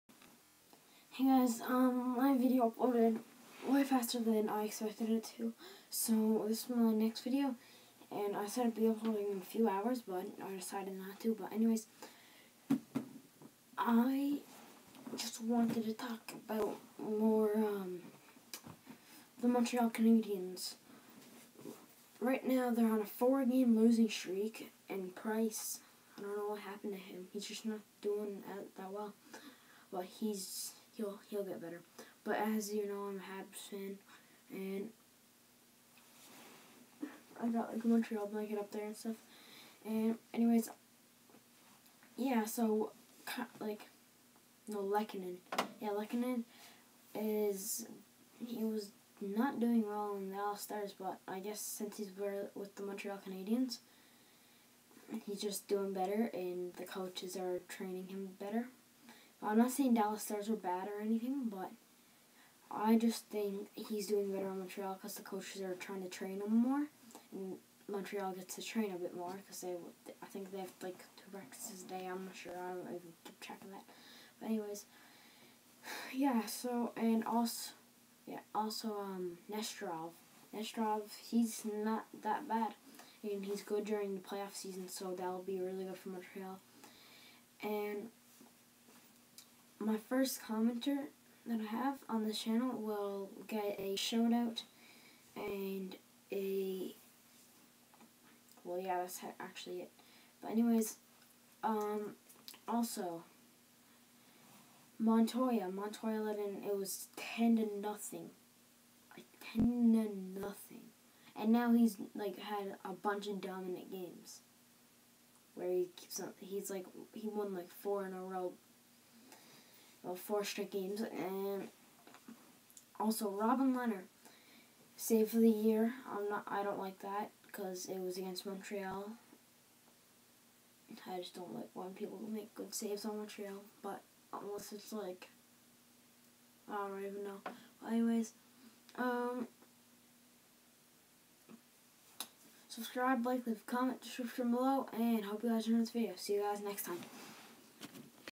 Do you think Lehkonen is good for Montreal?
Comment if you think Lehkonen is good for Montreal, I do don't know about you guys